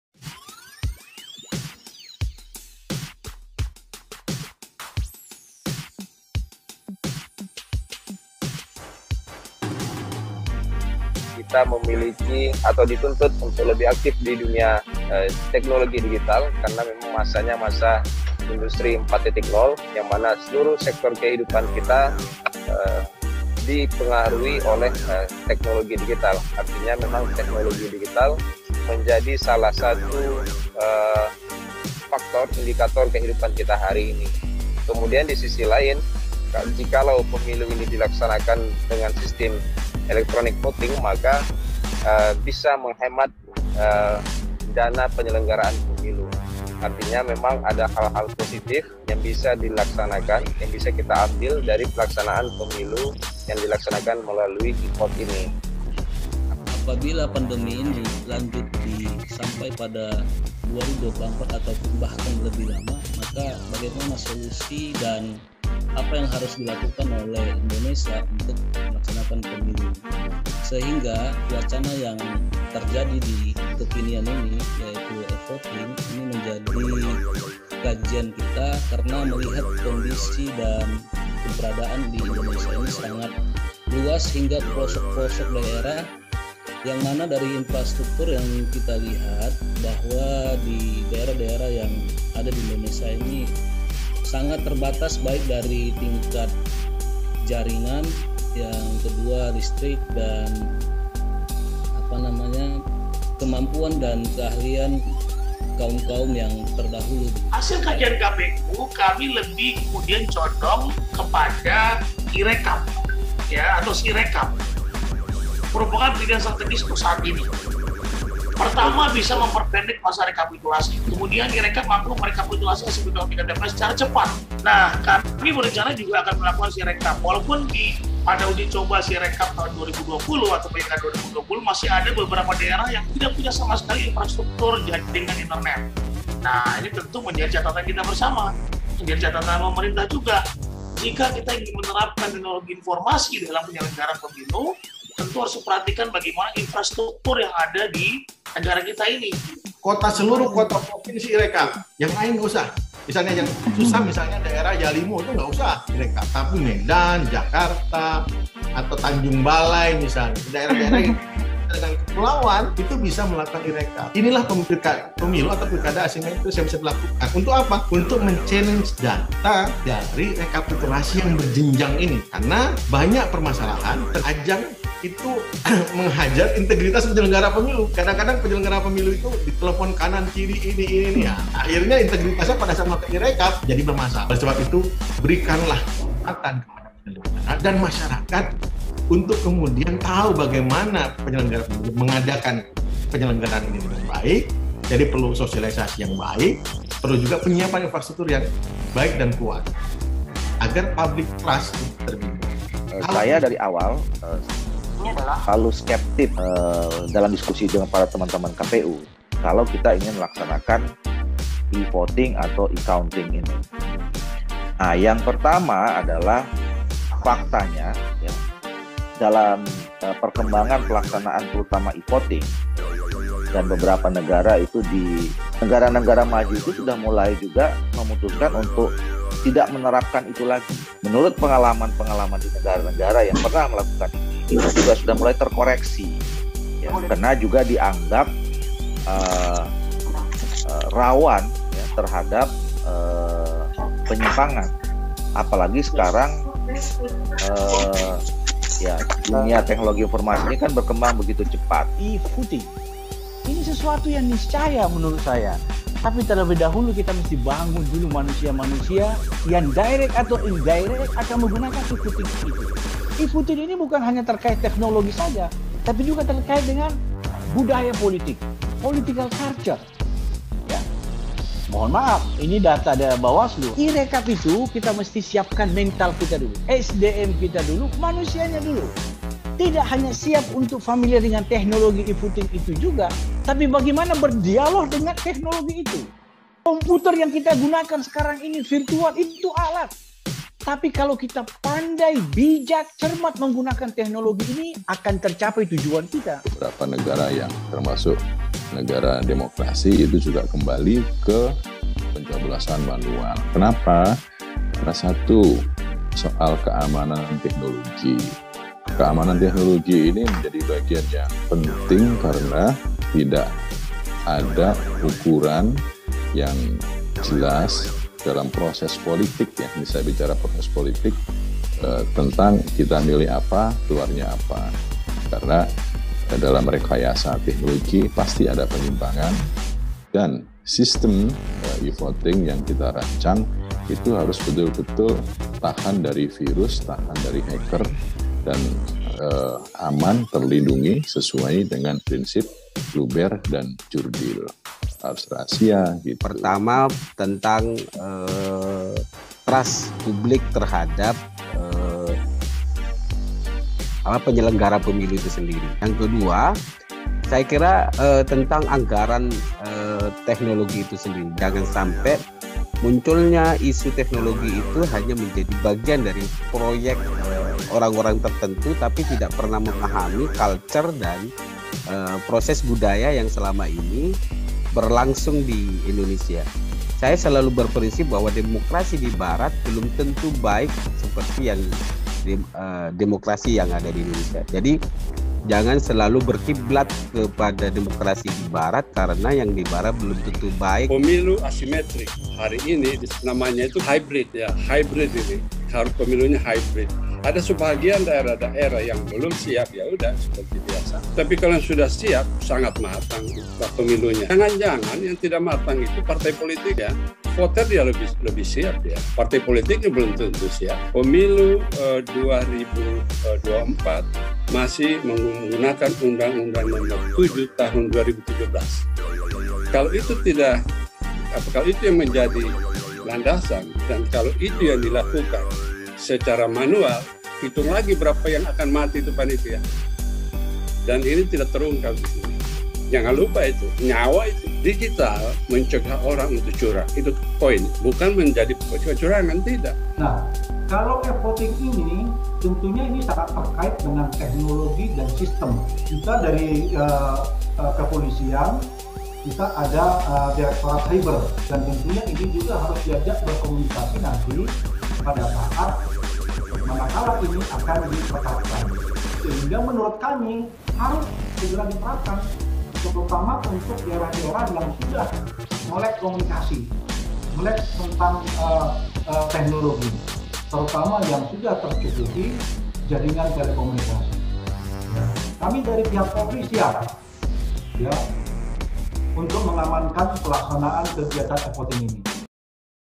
Podium Demokrasi PB HMI Bidang PDPP dengan Tema "Proyeksi Kesiapan E-Vote pada PEMILU di Indonesia"
Diskusi ini di laksanakan Oleh PB HMI bidang Demokrasi Politik dan Pemerintahan dengan narasumber Ilham saputra (Ketua KPU RI), Rahmat Bagja (Komisioner Bawaslu RI) , Ahmad Doli Kurnia (Ketua Komisi 2 DPR RI)  Dll.
moderator Acara Azmi Sitorus , diskusi ini dilaksanakan pada tanggal 12 Agustus 2021.